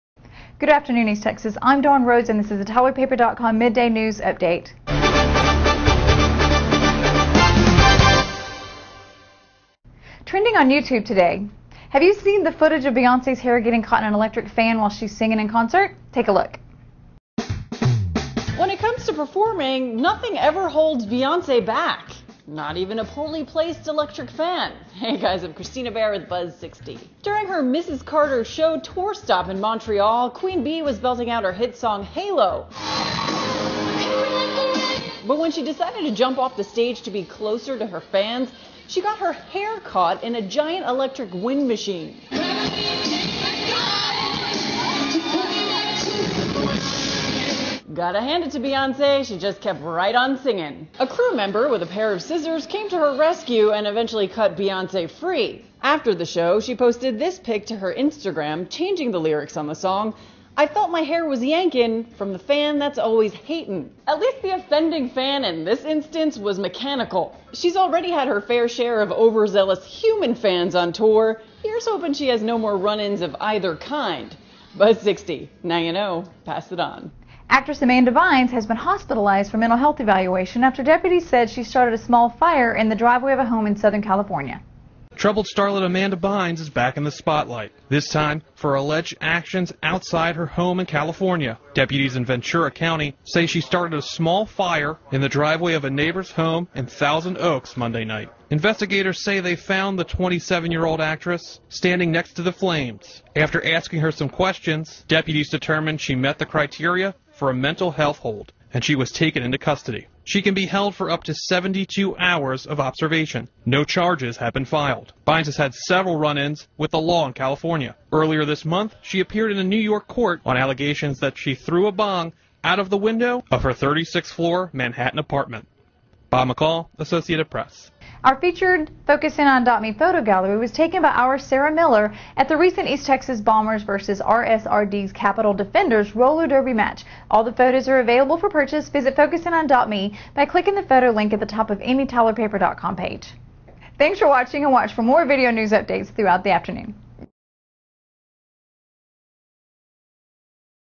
Tyler Morning Telegraph mid-afternoon news update for July 23, 2013
More faces in more places! Join our Megan Middleton, Brian Pearson and Dawn Rhodes for the video news roundup of what's happening at mid-afternoon!